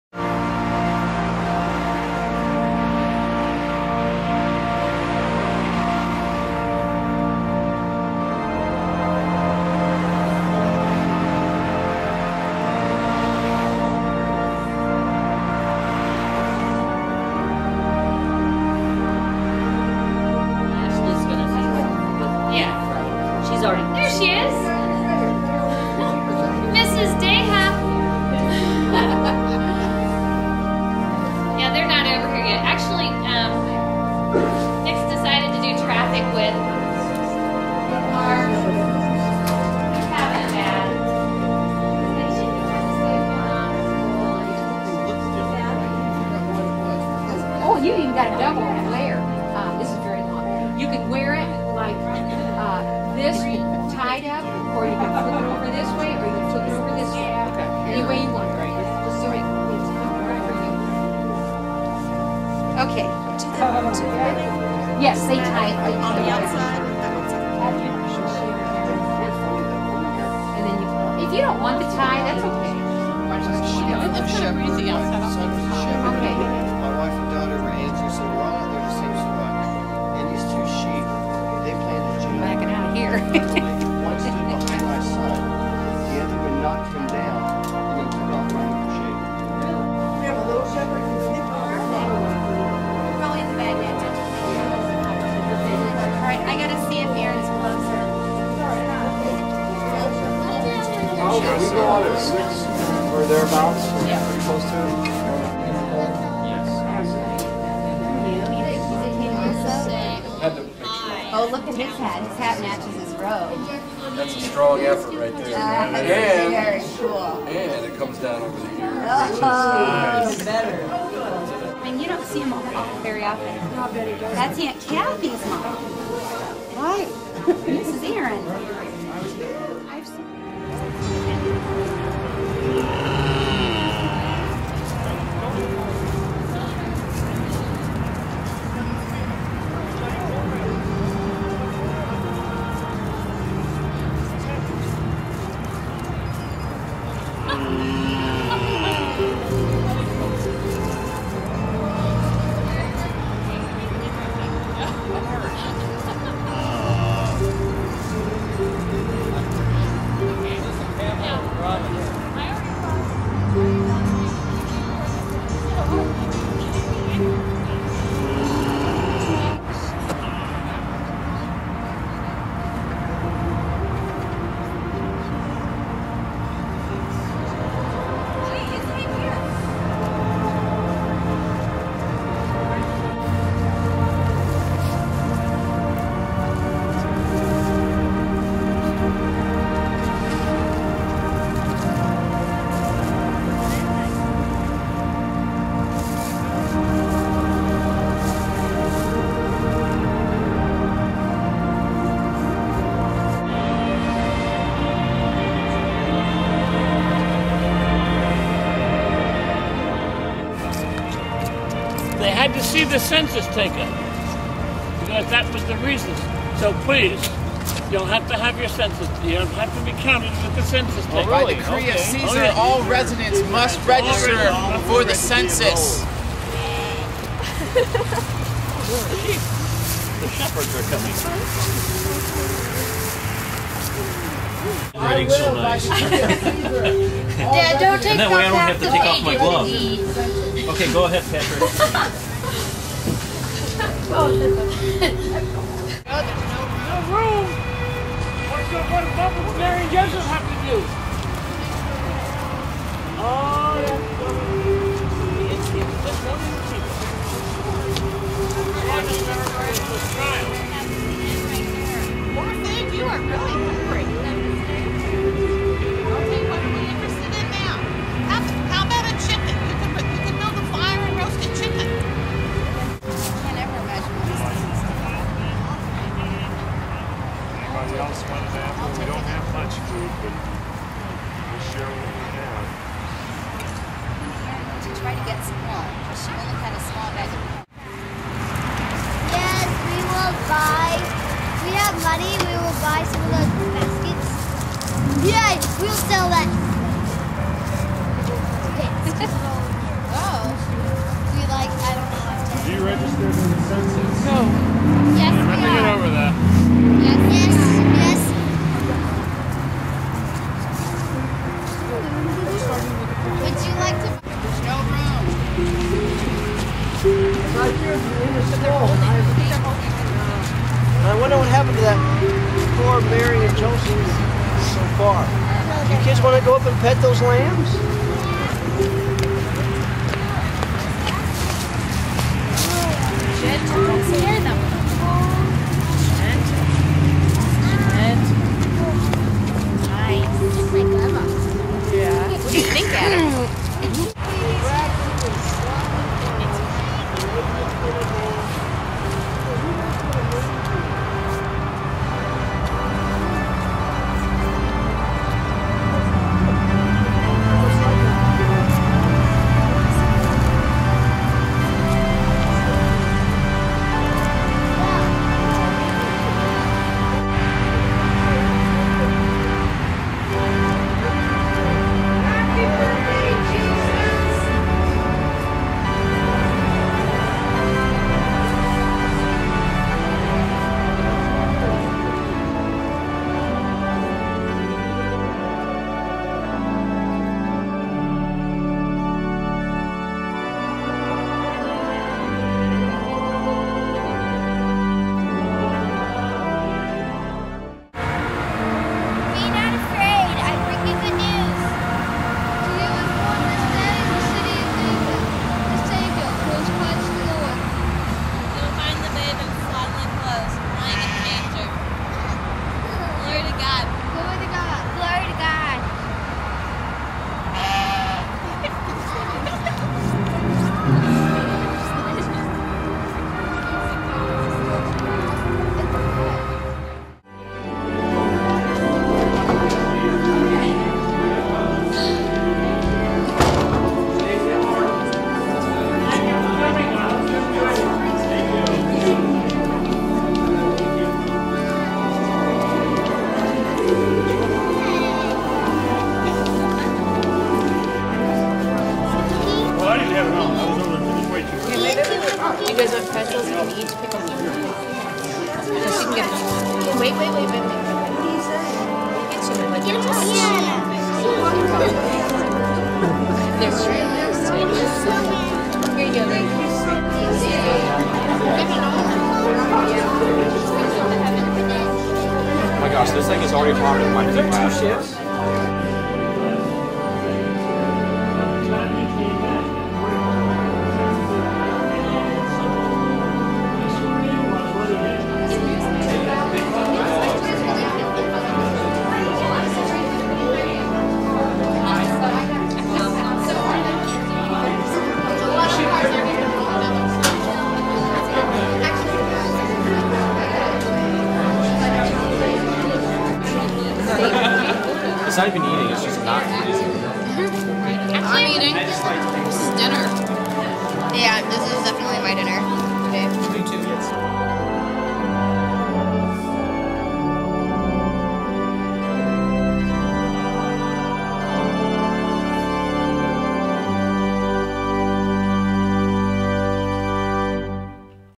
Live Nativity at Castleton United Methodist Church Dec. 2008
Complete with camels, sheep, donkeys, and goats, about 90 volunteers take part in a 2-night Live Nativity at Castleton United Methodist Church each December since about 1991.